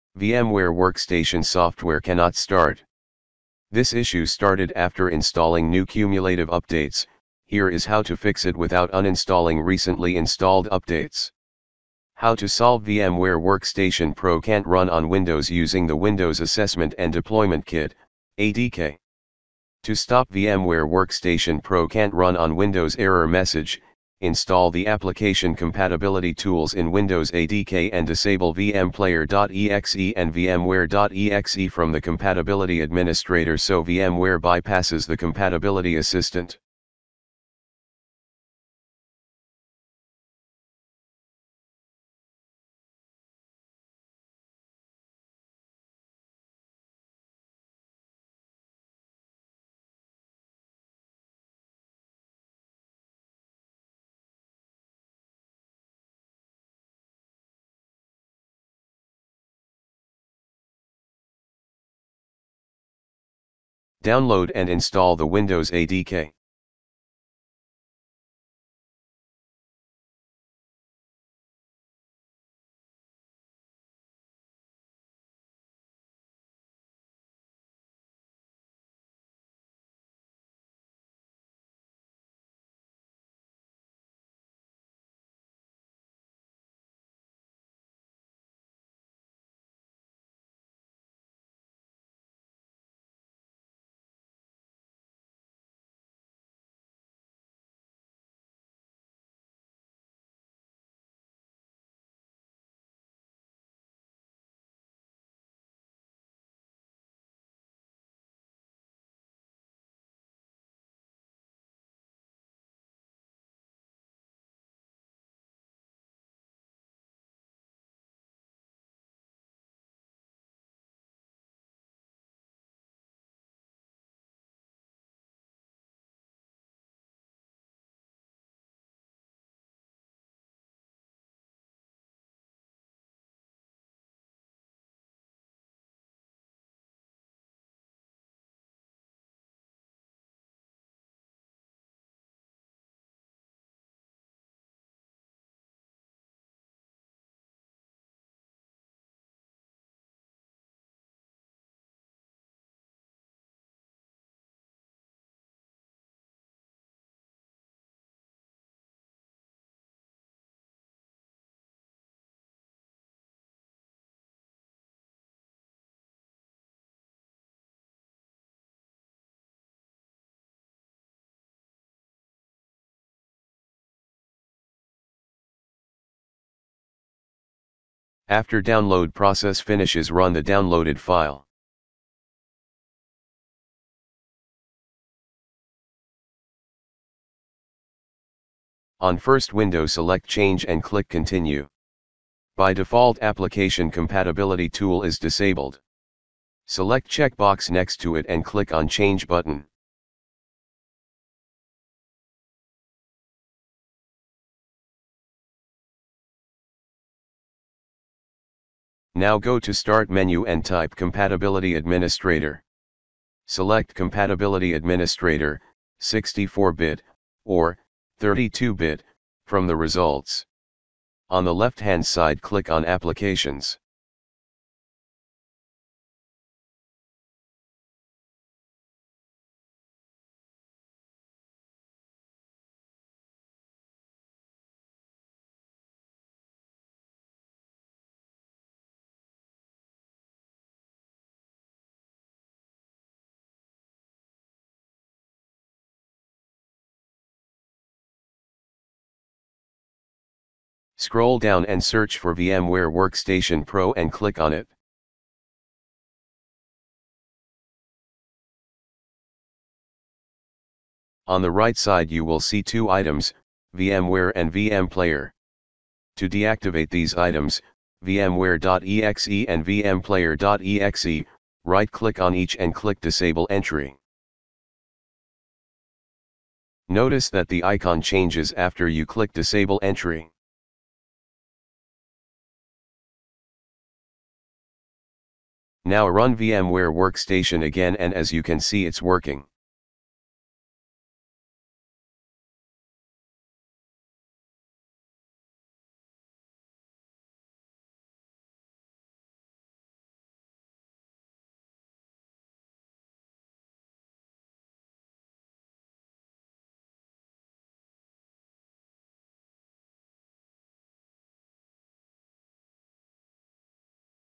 (Message Solved) VMware Workstation Pro Can't Run On Windows [ADK] (Without Uninstalling Updates)
VMware Workstation software cannot start. This issue started After installing new Cumulative Updates, Here is how to fix it without uninstalling recently installed updates.

How to solve VMware Workstation Pro Can't Run On Windows using The Windows Assessment and Deployment Kit (ADK).
To Stop VMware Workstation Pro Can't Run On Windows Error Message, Install the Application Compatibility Tools in Windows ADK and disable vmplayer.exe and vmware.exe from the Compatibility Administrator so VMware bypasses the Compatibility Assistant.
Download and install the Windows ADK.
After download process finishes run the downloaded file.
On first window select change and click continue.
By default application compatibility tool is disabled. Select checkbox next to it and click on change button.
Now go to start menu and type "Compatibility Administrator".
Select "Compatibility Administrator (64-bit) Or (32-bit))" from the results.
On the left hand side click on applications.
Scroll down and search for VMware Workstation Pro and click on it.
On the right side you will see two items (Vmware and Vmplayer).
To deactivate these items (Vmware.exe and Vmplayer.exe), Right click On each and click disable entry. Notice that the icon changes after you click disable entry.

Now Run Vmware workstation again and as you can see its working.